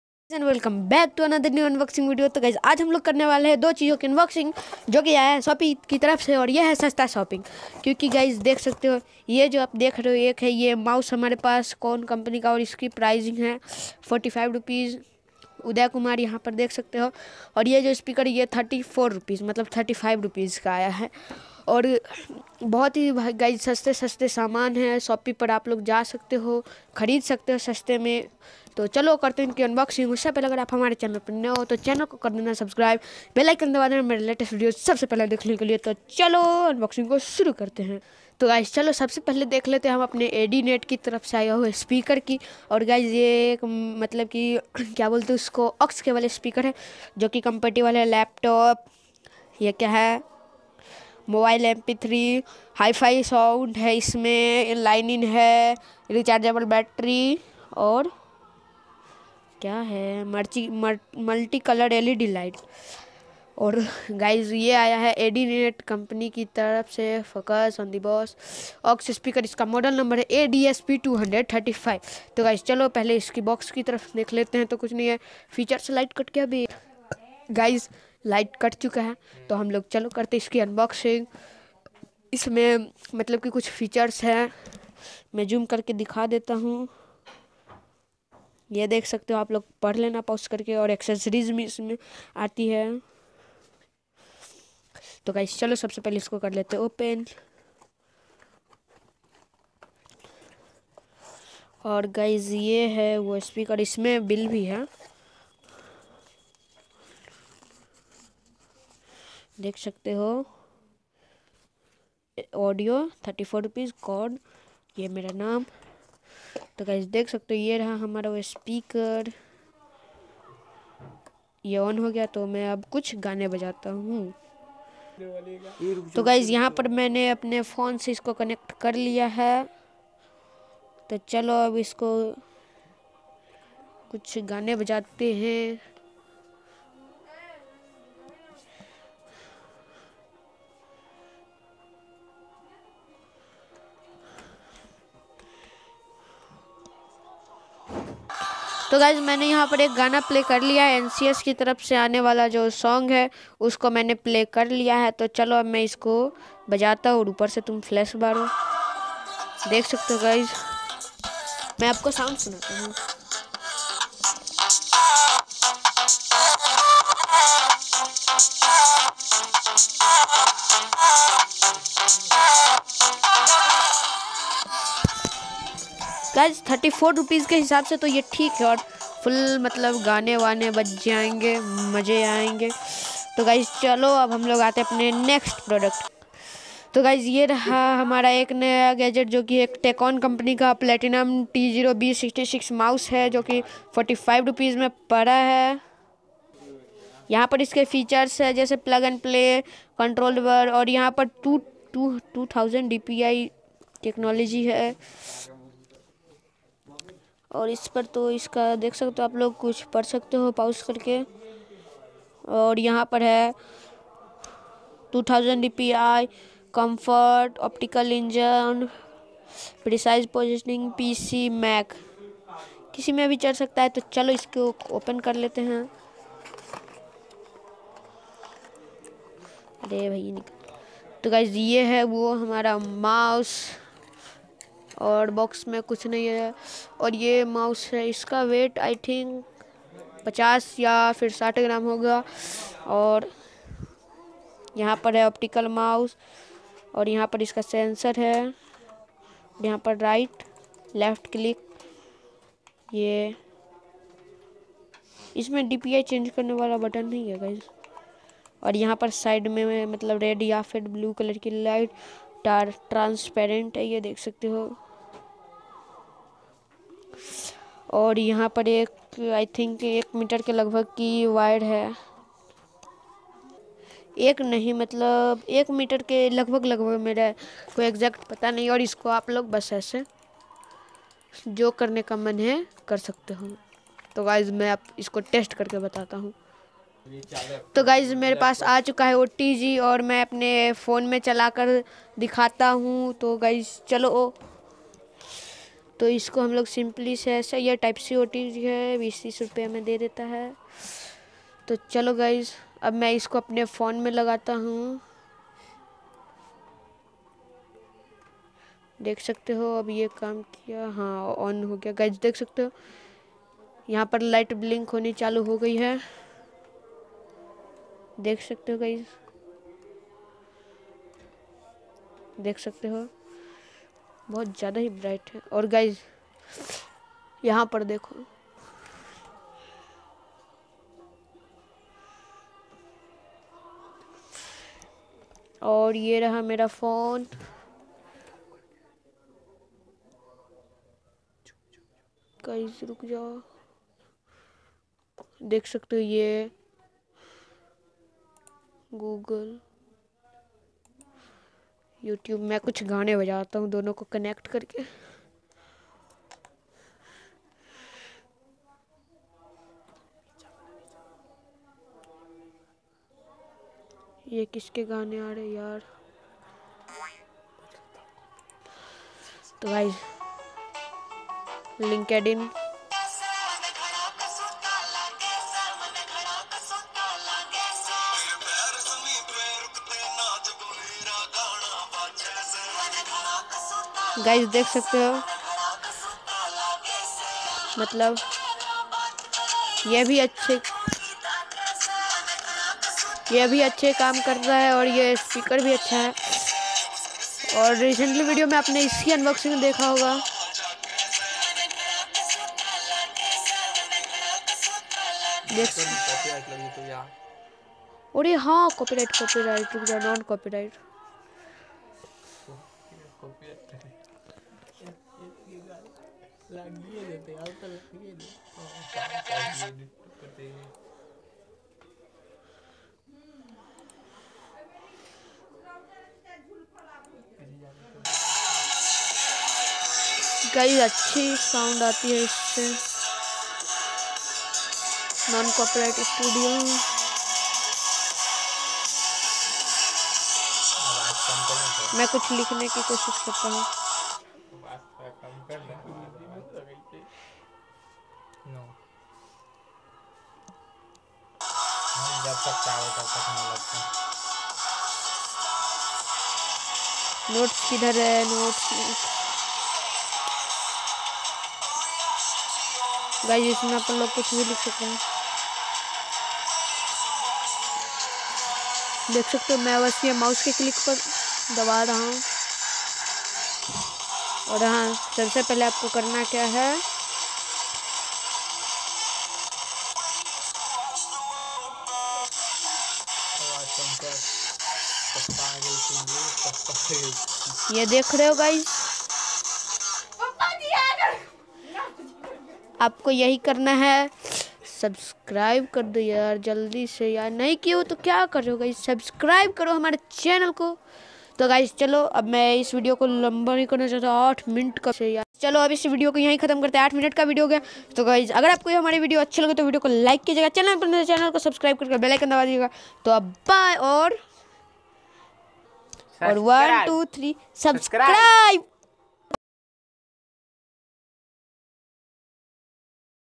SHOPEE APP SASTA SHOPPING UNBOXING AND REVIEW 🔥BEST PRODUCT FROM SHOPEE 🔥 ASTRA'TECH
SHOPEE APP SASTA SHOPPING UNBOXING AND REVIEW OF A SPEAKER FOR RS 34
AND A MOUSE FROM RS 45

IN THIS VIDEO I AM UNBOXING AND REVIEW OF THESE SHOPEE APP SASTA SHOPPING